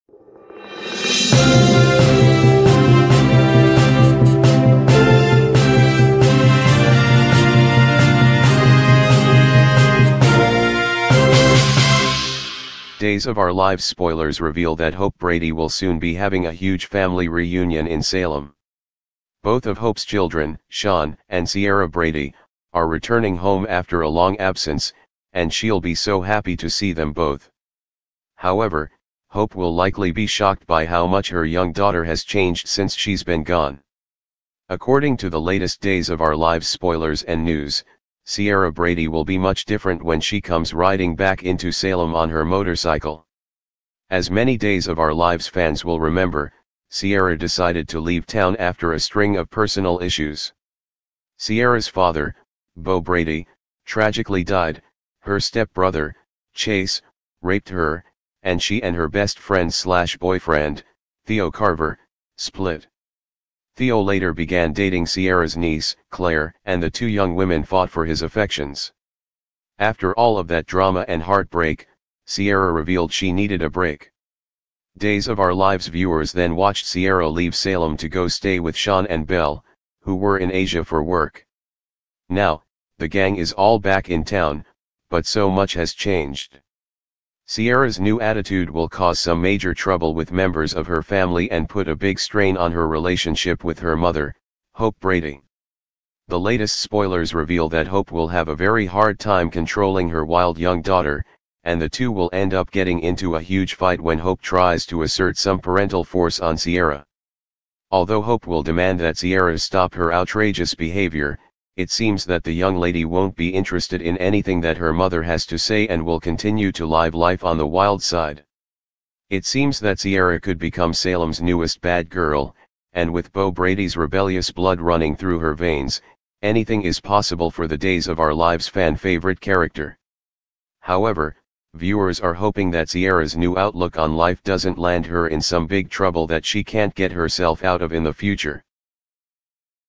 Days Of Our Lives Spoilers: Hope will be stunned by her daughter's wild new behavior
Days of our Lives spoilers reveal that Hope Brady will soon be having a huge family reunion in Salem.